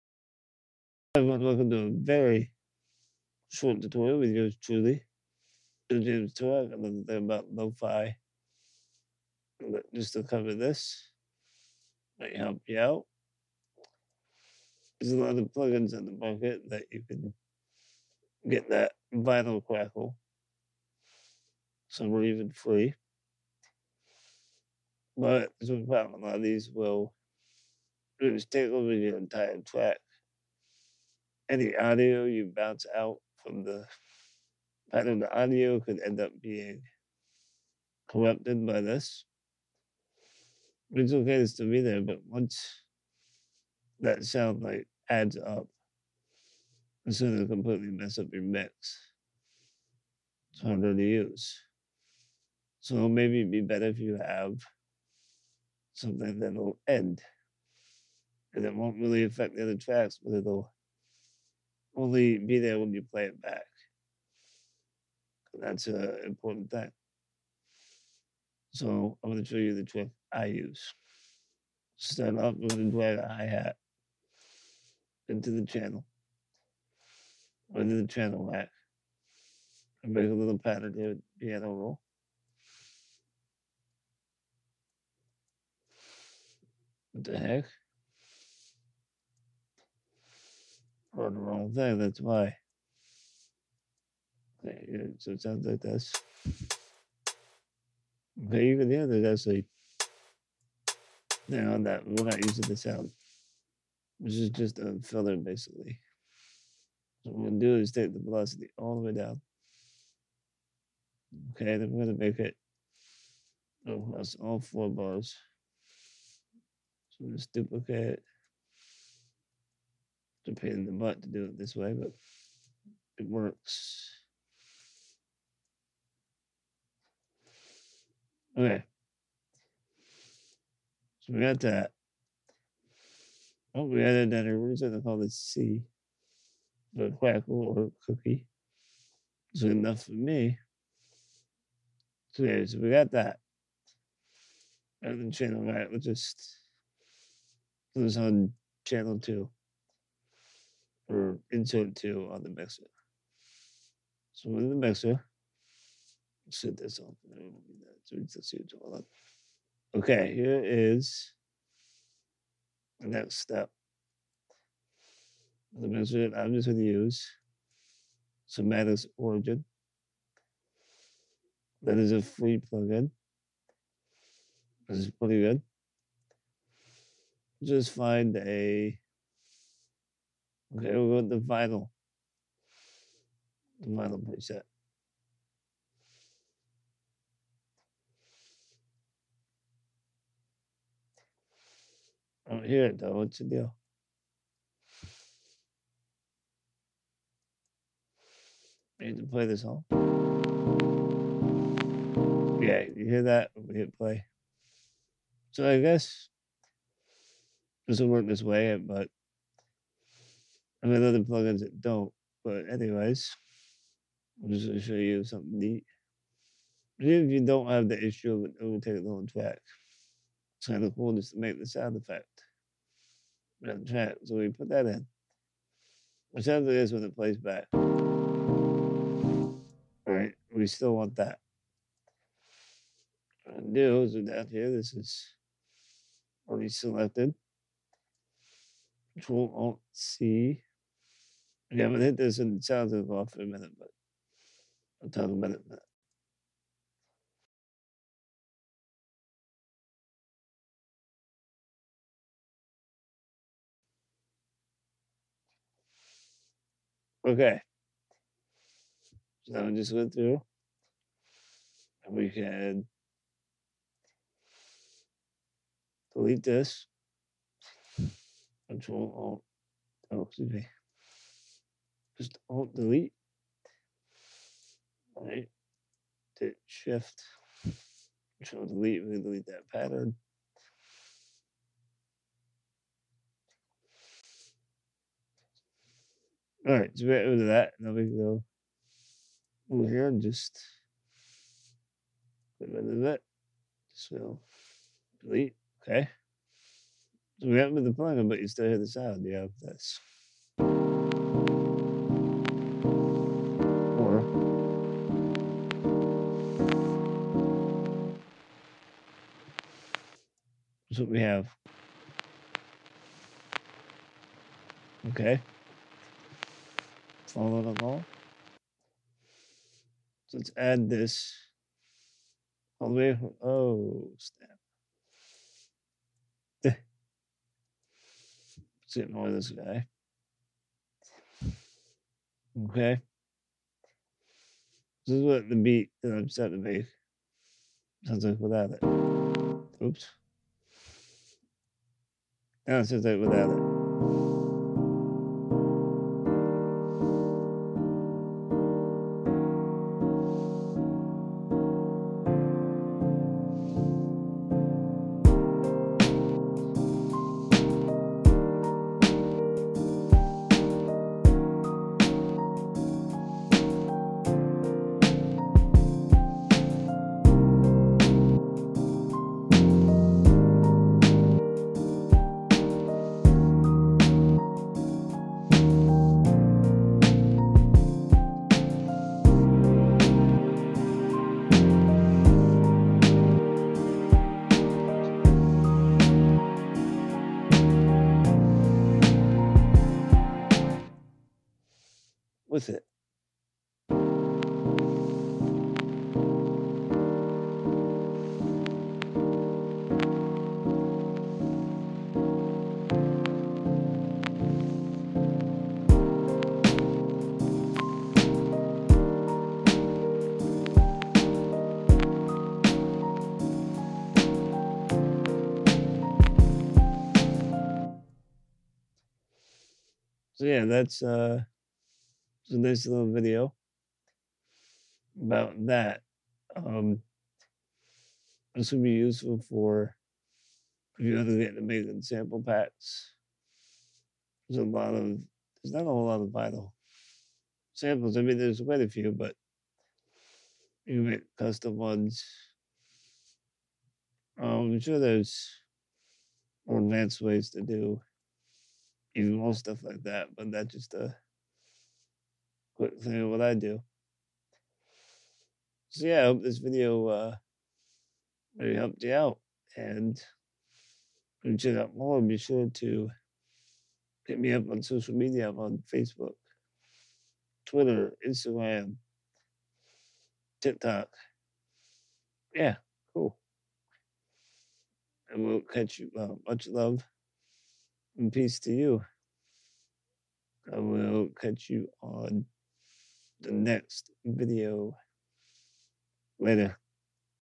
How to Lofi Crackle
This is my trick for getting that signature lofi hip hop vinyl crackle.